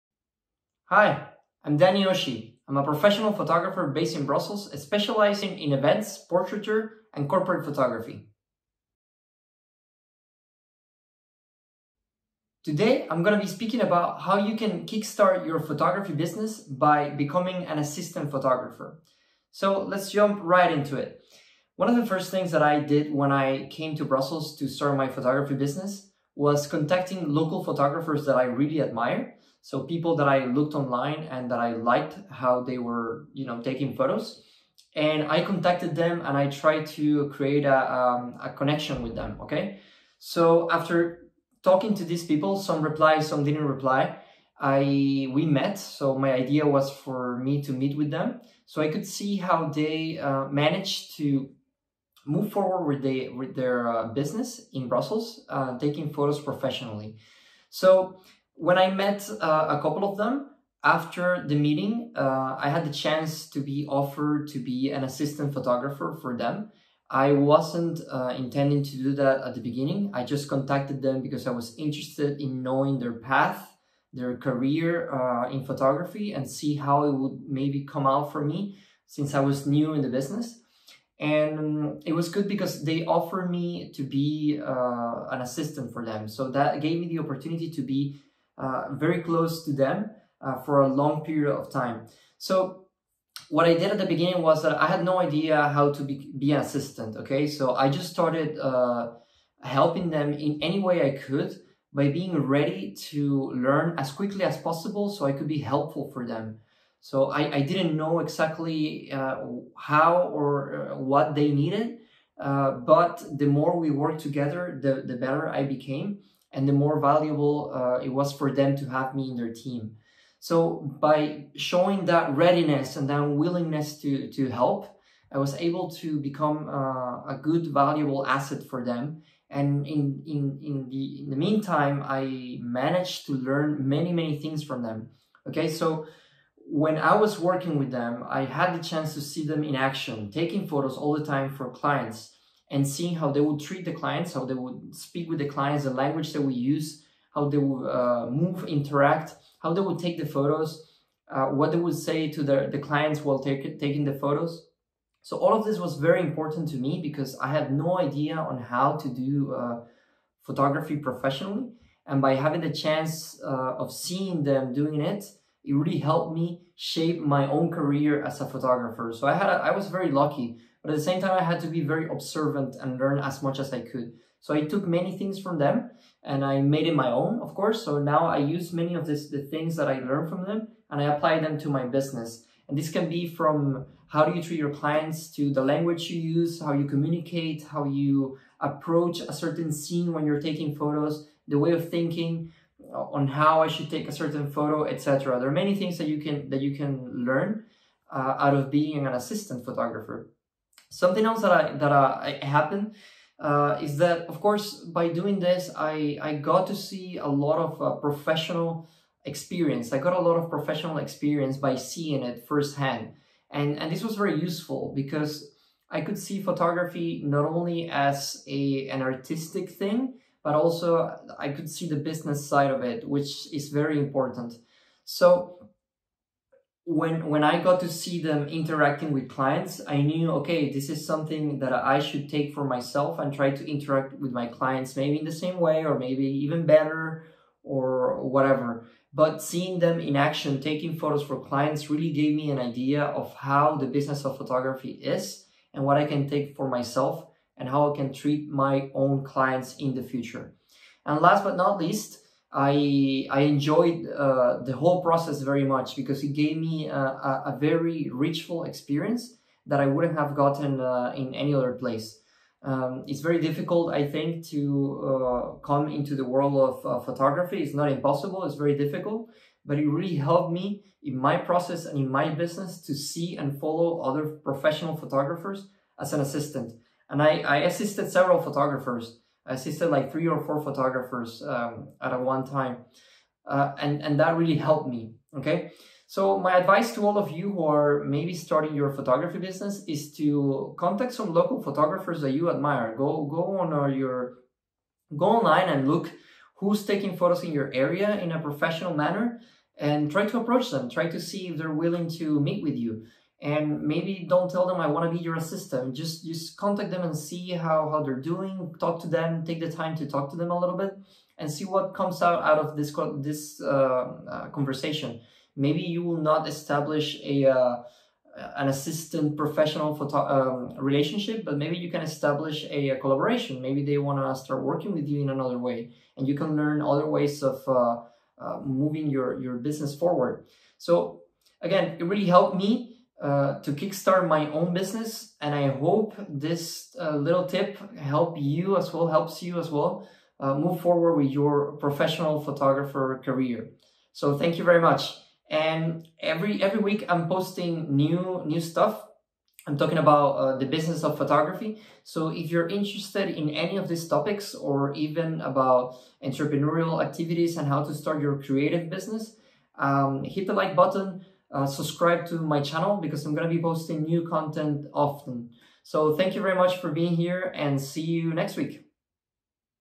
Kickstart your photography career by becoming an assistant photographer
Becoming an assistant photographer was highly beneficial to my career as a professional photographer that taught me many things and accelerated my success. Having that first-hand experience was extremely valuable in my early days, and I still apply many of the things I learned in my own photography business.

If you are curious about my photography and what I do professionally, please check my